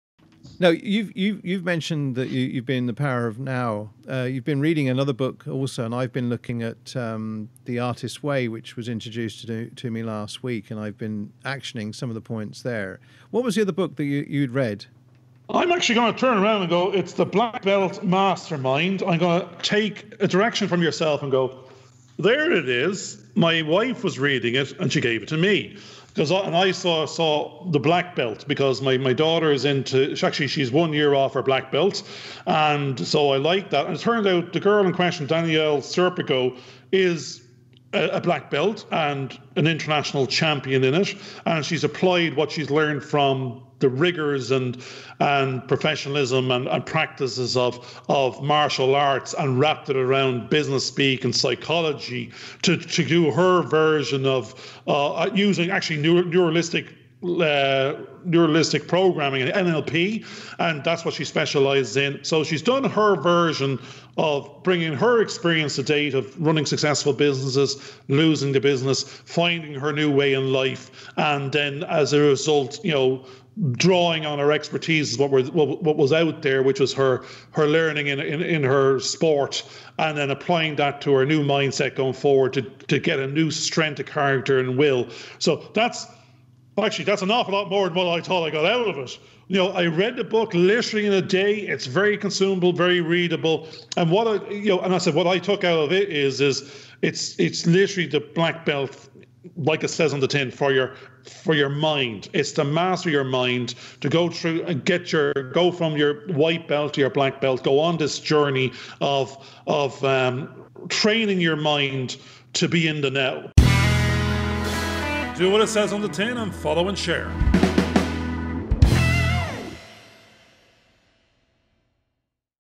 Listen 6
Garvin just read another self help book, God help us all, we are going to have to listen to him make a dogs dinner of it, he needs all the help he can put his hands on and quick, he is running out of Now's and he still hasn't figured anything out yet.
"The Power of Now" summed up in 2 words - "Just Be", He waves another one at George "The Black Belt Master Mind", he explains he doesn't even have a white belt, can he somehow just get the other belts by osmosis as learning is not his forte.
The Jokers are ping ponging different authors at each other in a battle of 3 Points, 3 Principles or 5 Big Things, forty love who knows, but one thing is for sure, they need a plan, they need to write it down, then do something about it and that does not mean sticking the list up the chimney for Santa Claus to deliver on because they will be waiting awhile for that one to come to fruition.
George is taking his inner child on regular artist dates now, he no longer hears the voices in his head they are now external, I hope he isn't seeing dead people, but I am happy that he now has someone else to talk to other than his inner chatterbox.
The Lads have difficulty prioritising items on lists, buy the milk or change careers, they will have to get up that extra hour early in the morning to adopt "The Millionaire Mindset" or even better ask the milkman if he is looking for an Intern. They have decided to do both.
George wants to talk about a Bin Full of Balls so he warns Garvin to "Shut up and Let me Talk", OK that's Garvin's words but we all know that's what he meant. The Bin is finite but also as Big as the Universe Go Figure.
The Jokers realise the mistake they are making is trying too hard, Who Knew all you have to do is Sleep on it, all the answers you need will be there when you wake up. Garvin then remembers that he actually works harder in his sleep than when he is awake, there just not billable hours to his annoyance. George reminds Garvin Listening involves not talking and in particular him not talking so a Good Conversation can be had by all, unfortunately Garvin didn't hear him.